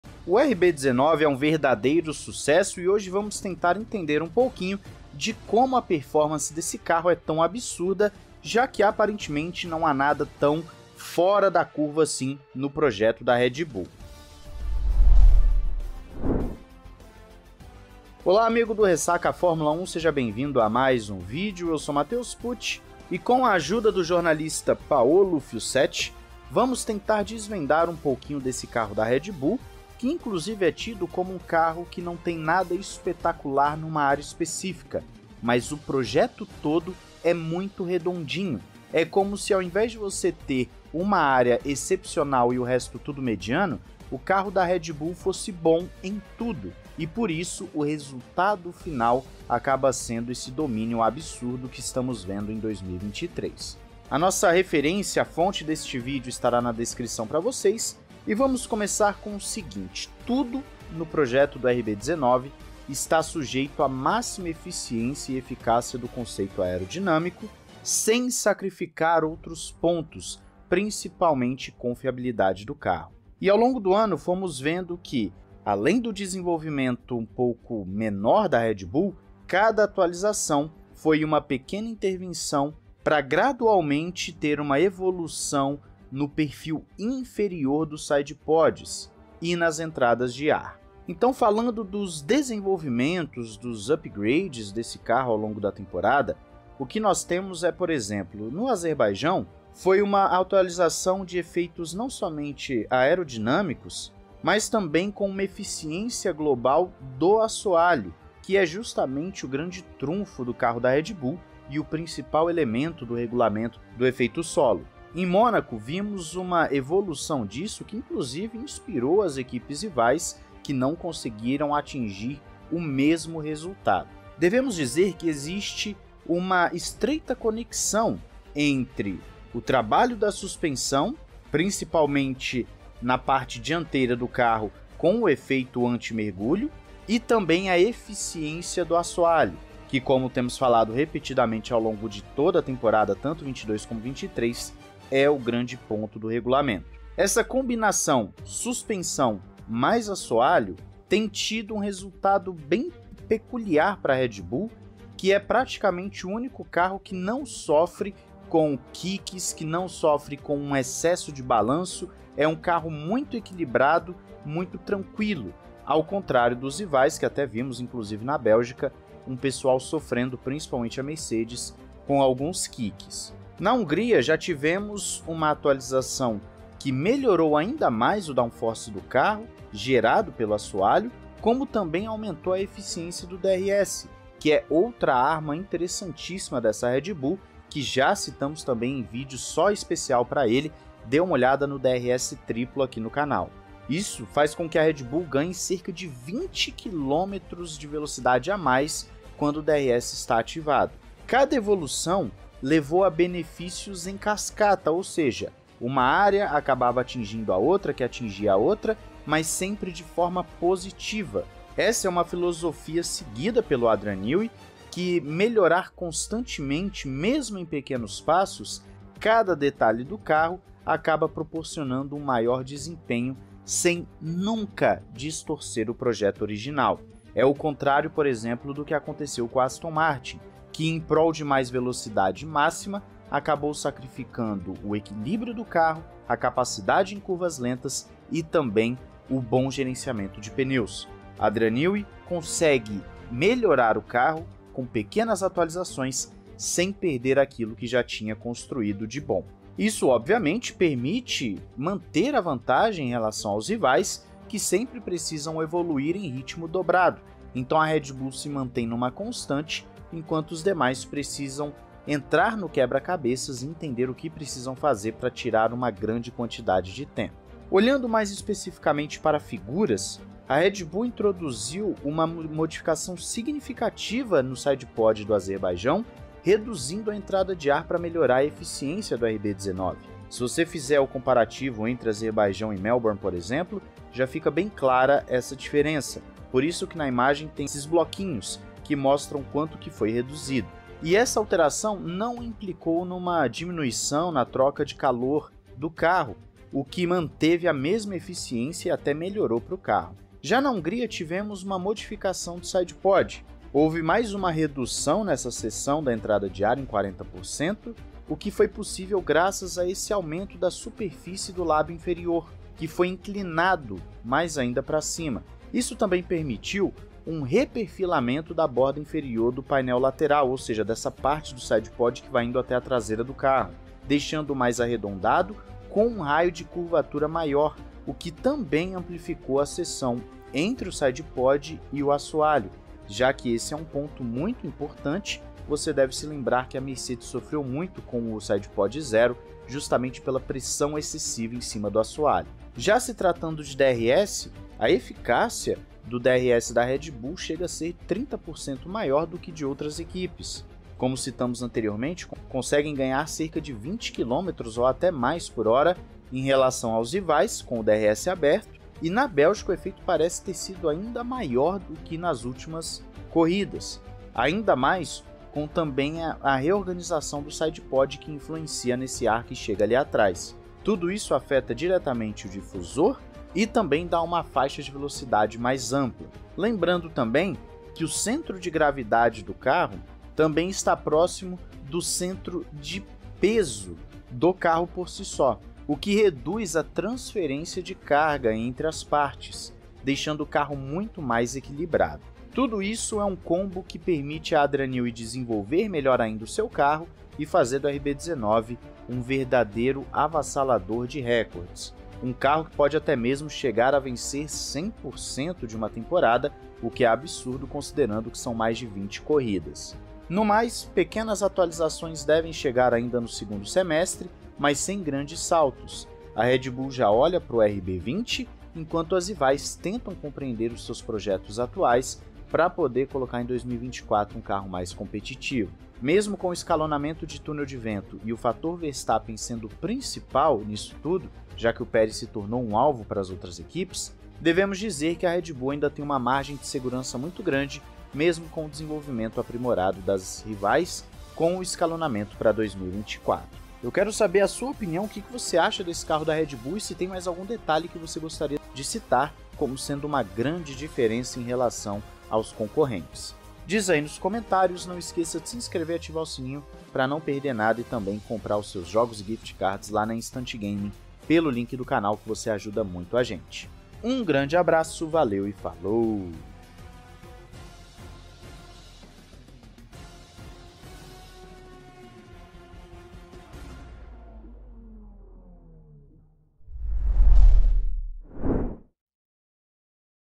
OS SEGREDOS DO RB19 QUE MOSTRAM A GENIALIDADE DE ADRIAN NEWEY E JUSTIFICAM O DOMÍNIO
Sergio Milani
@scuderiamilani (Twitter)

João 3:16

00:00 - Introdução
00:58 - Cada ponto conta
03:21 - Efeito cascata no carro
04:27 - Ilustrações para compreender
06:42 - Avassalador de recordes